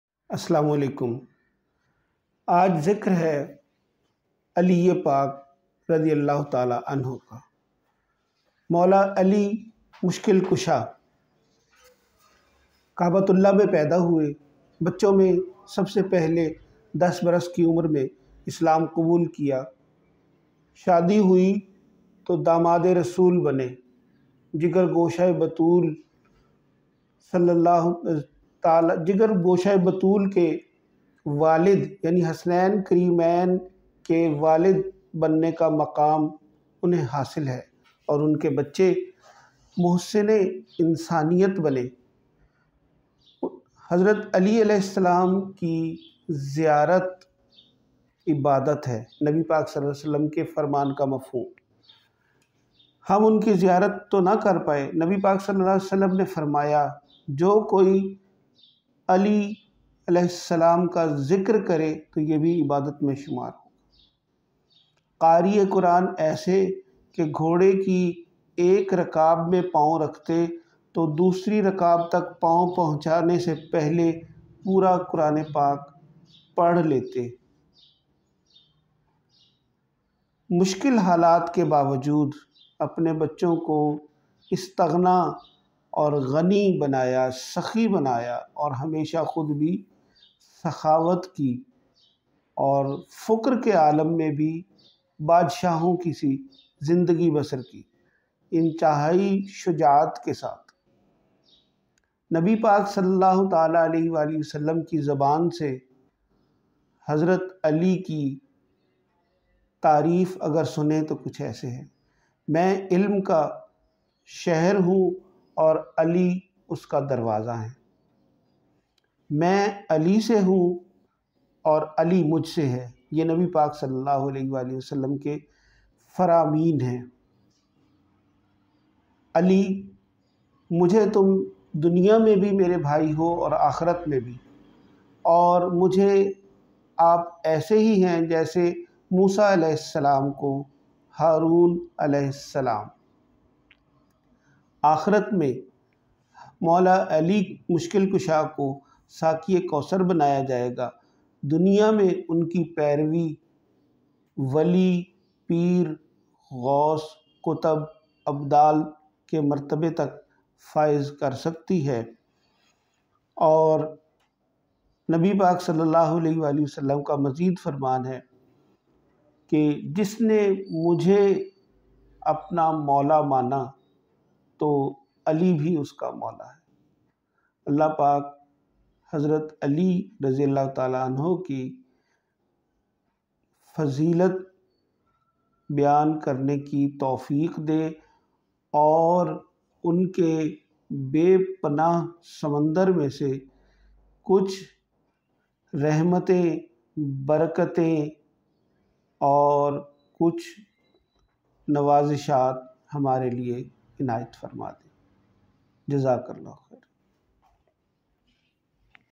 Maula ALI
Sayings Of Holy Prophet for Hazrat Ali alaih e Salam to make us understand his greatness are described here .